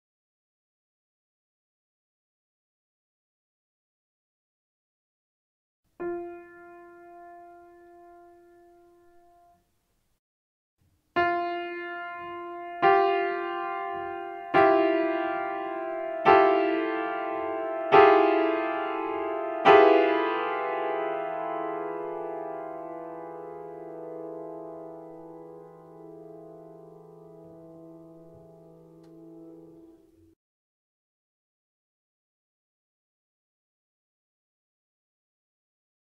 Cluster, lesson 64_Cluster, Aufgabe 64 | Ear training | Gehörbildung
Cluster

In this lesson you have to figure out a cluster, which is building up note by note – with every attack one note is added. You only have to write the notes, which are added and not the whole chords. Good luck!

Cluster

In diesem Aufgabentyp hört ihr ein Cluster, das Ton für Ton aufgebaut wird – mit jedem Anschlag wird ein Ton hinzugefügt. Ihr müsst nicht jeweils den ganzen Akkord notieren, den ihr hört, sondern nur den Ton, der bei jedem Anschlag dazukommt. Viel Erfolg!